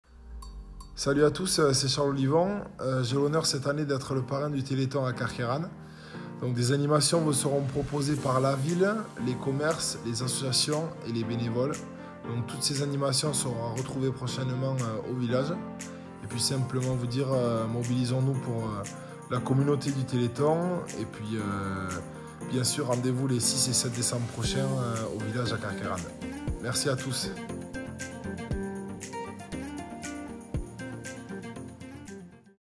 Charles Ollivon, parrain officiel du Téléthon 2019 de Carqueiranne
RDV les 6 et 7 décembre prochains !